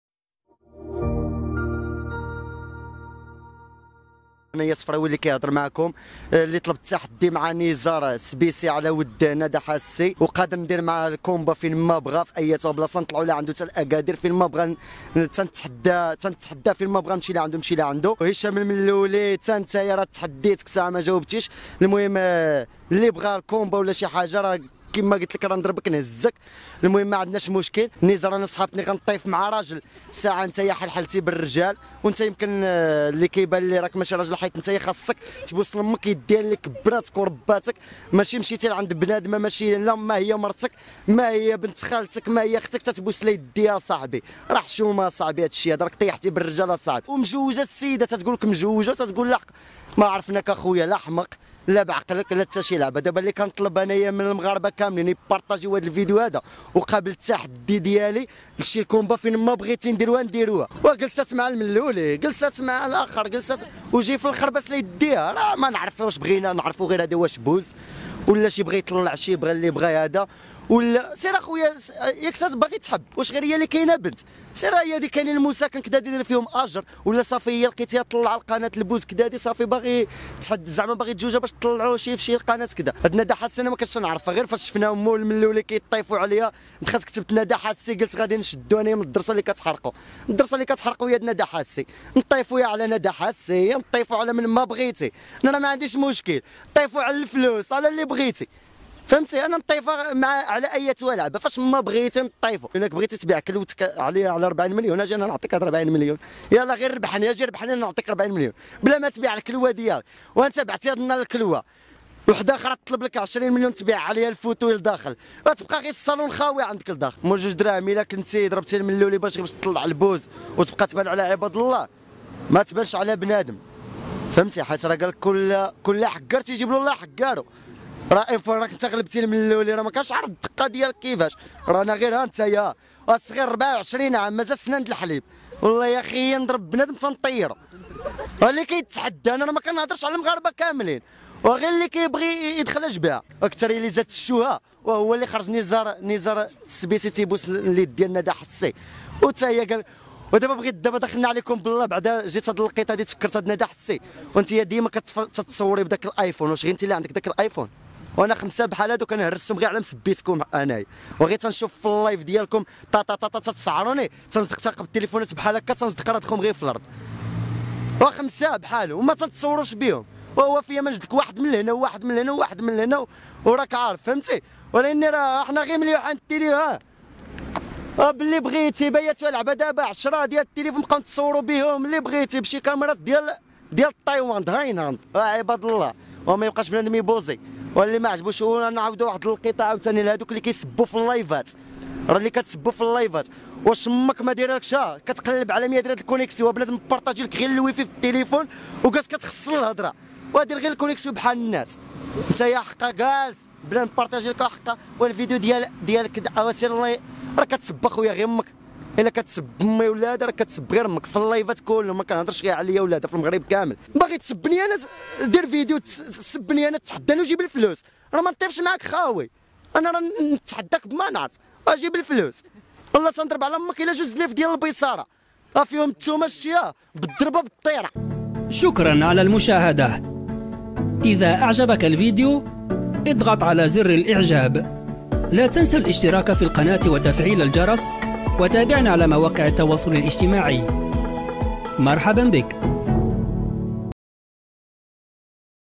صفريوي دخل طول وعرض ف هشام الملولي و ندى حسي نزار سبيتي. نخسر علييكوم 2 زلايف البيصارة
2. اضغط على الجرس (🔔) بجانب زر الاشتراك
3. شارك مقاطع الفيديو  مع أصدقائك وعائلتك